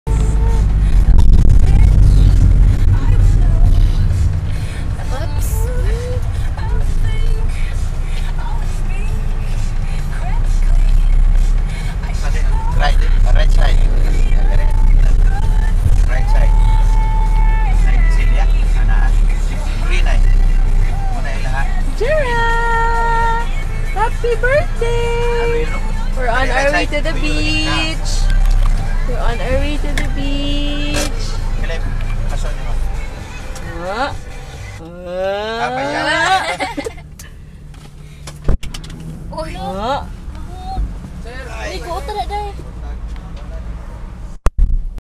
Jera Mae's 1st birthday (April 24, 2010)
It was Jera Mae's birthday when this video was taken. We were on our way to the resort on an early, sunny morning. As usual, Jera was acting like a good girl, just sitting down, while wearing her new shades given by her Auntie Doy. :-)